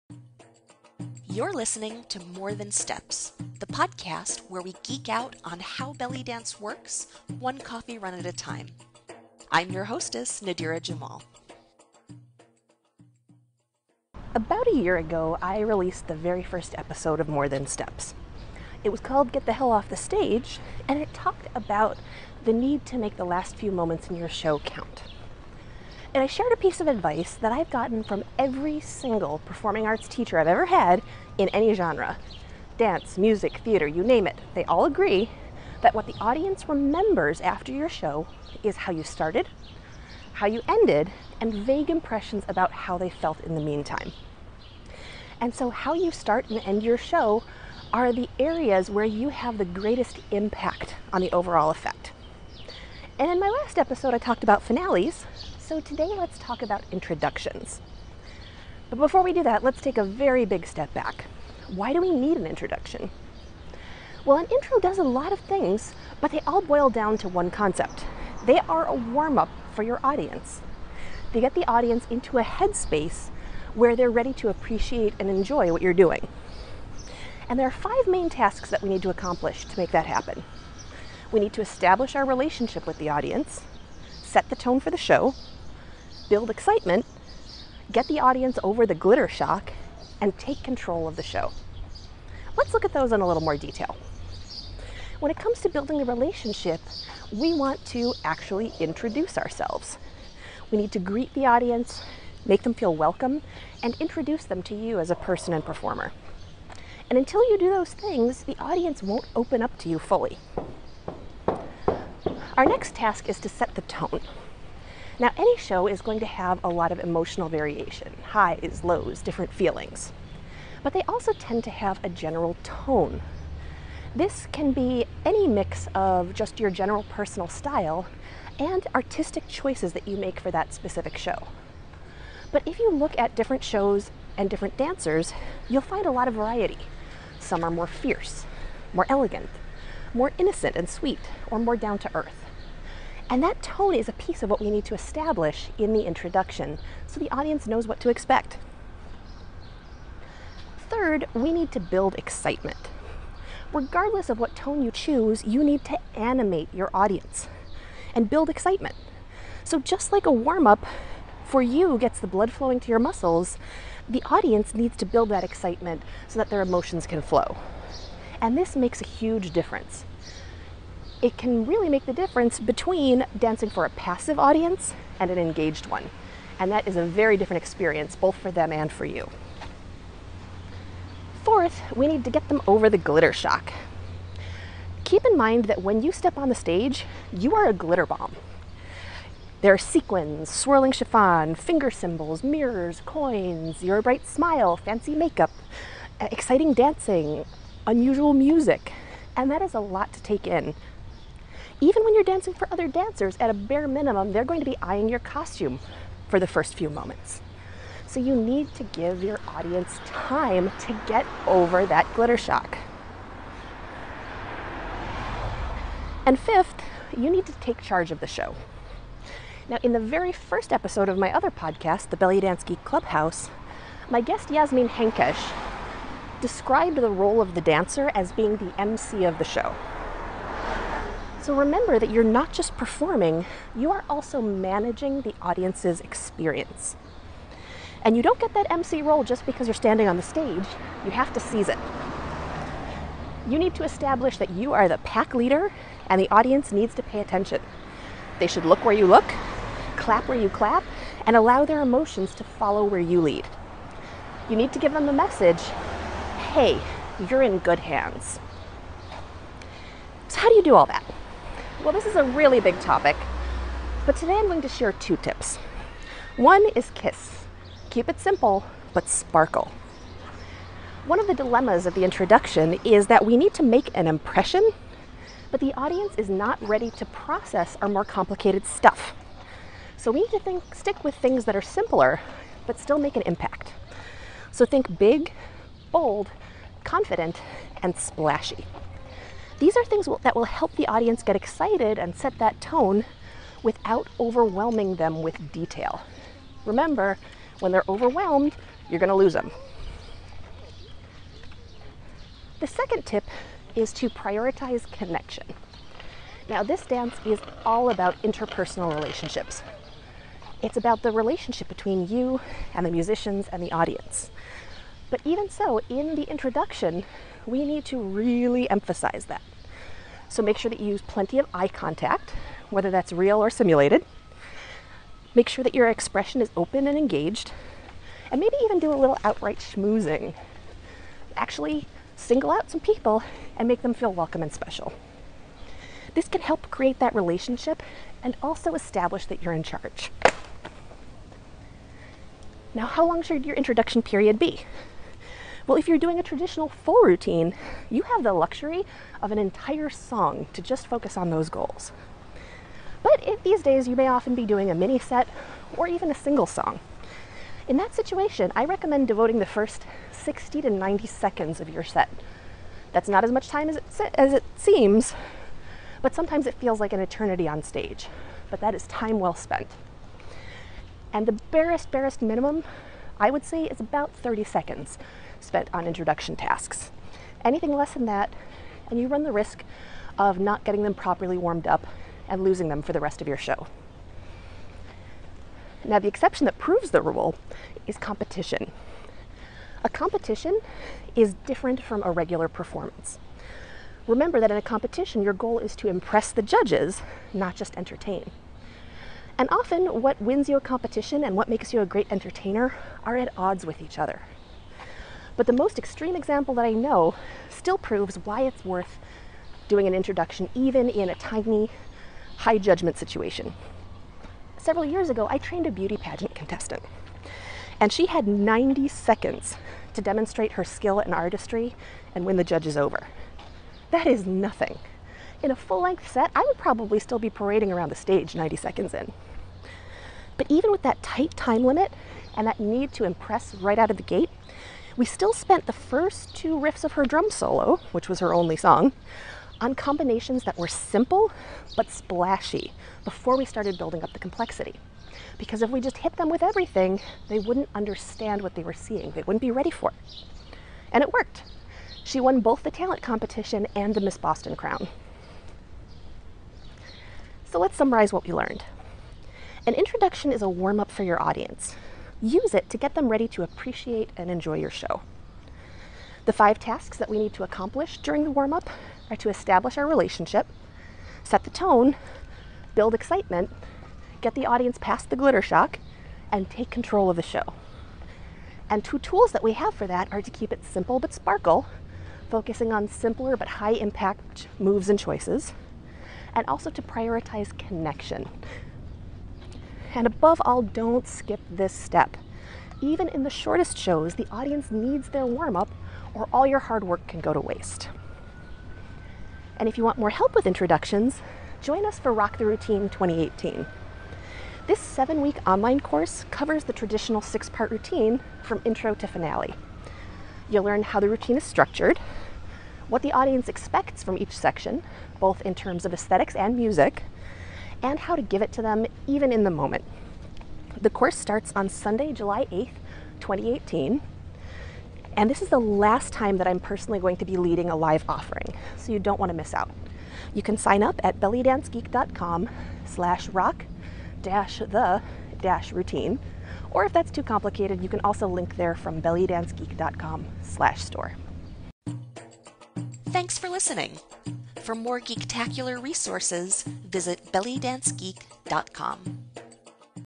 More Than Steps Episode 43 - Warm Up Your Audience
More Than Steps -  The podcast by Belly Dance Geek

5 things your introduction needs to accomplish